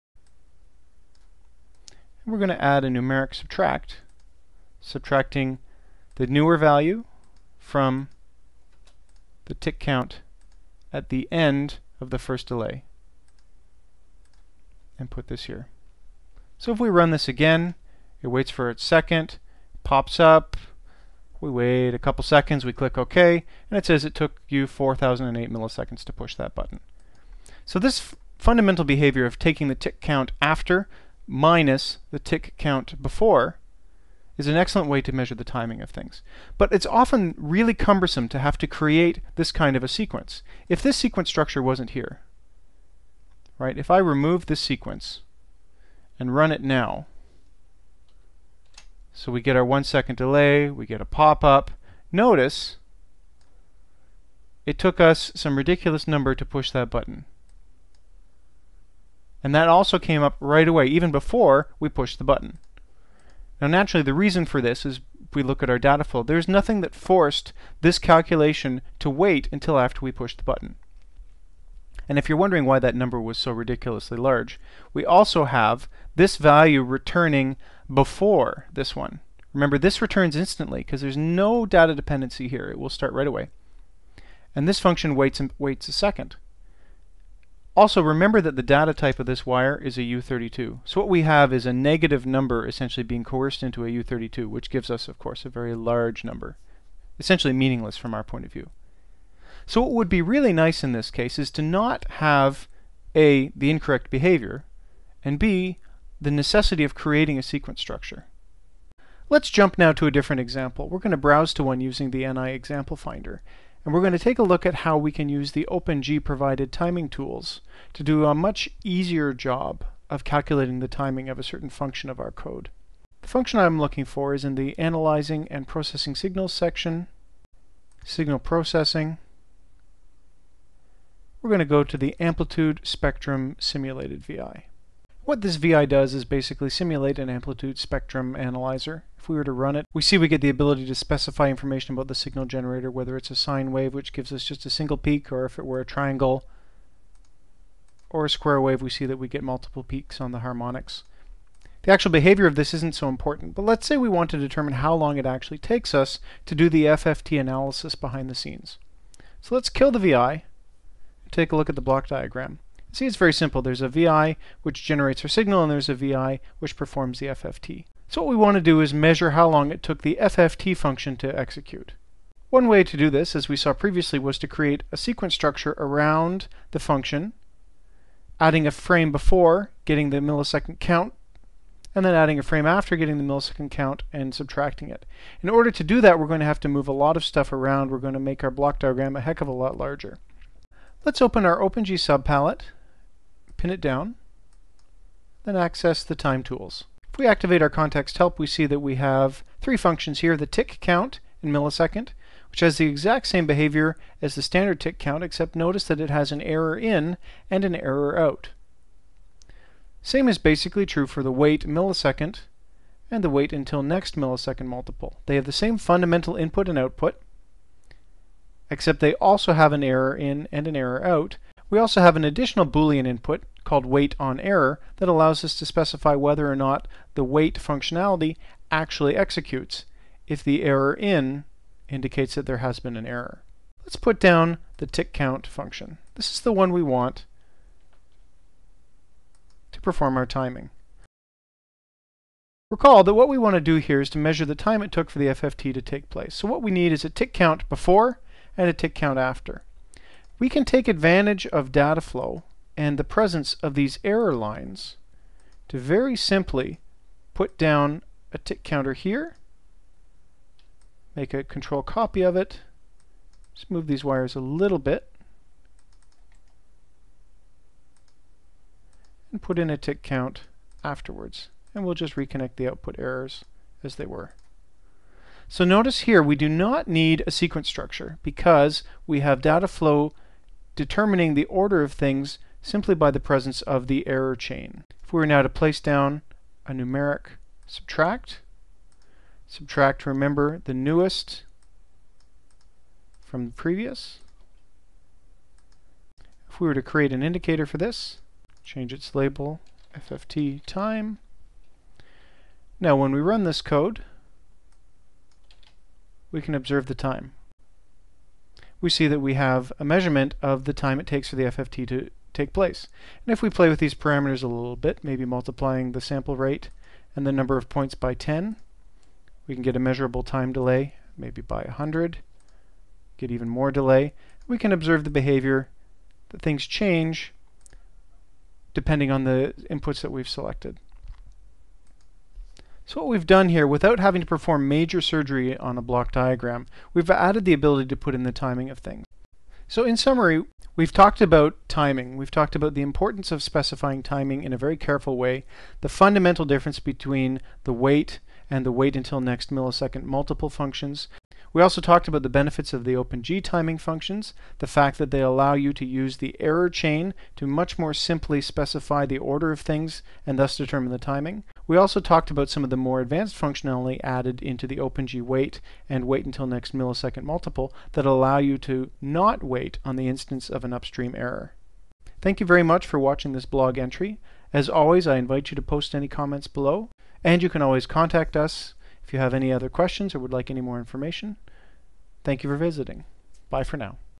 LabVIEW Blog - Part 2 It's All About Timing
In this entry, I discuss what is often one of the most challenging issues in programming with LabVIEW: Timing.

Speaking from personal experience, problems with timing are among the most common sources of bugs for new LabVIEW programmers. I often hear complaints along the lines of: "I get this weird behavior, but when I run with Execution Highlighting turned on, it always works." Or, "I have to run my VI twice in order to get everything to initialize properly."

These are usually symptoms of timing issues which can be very difficult to debug. Good practices, and carefully watching your dataflow for unintended parallelism are the only real solutions to developing good, trustworthy code.

In this blog entry, I touch on these issues, and also spend time discussing another aspect of timing within LabVIEW: measuring and adding delays within your LabVIEW code, using:
 Tick Count (ms)
 Wait (ms)
 Wait until next ms multiple
 The OpenG Enhanced Timing Functions

As always, please feel free to add your comments below.

Ben Zimmer
-- LV Mastery Team